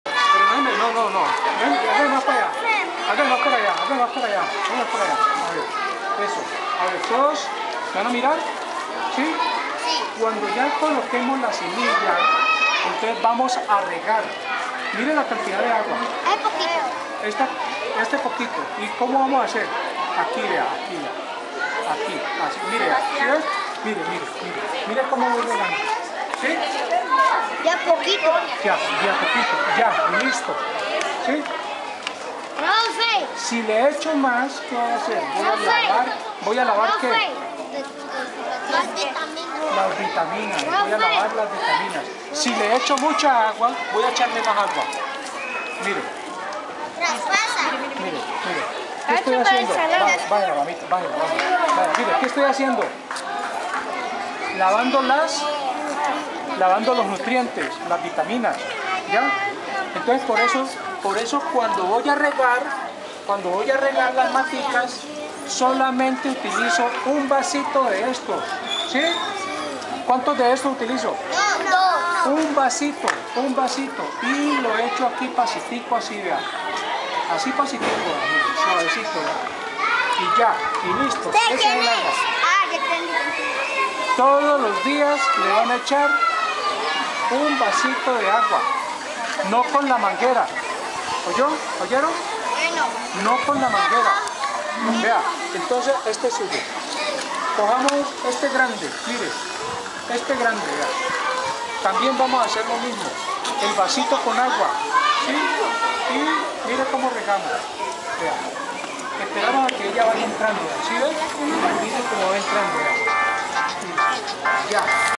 LOS NIÑOS DE SILOÉ SEMBRANDO FUTURO 2
EL TRABAJO EN LA DEFENSA Y CONSERVACION DEL MEDIO AMBIENTE SEGUIRA CON LOS NIÑOS DE SILOÉ.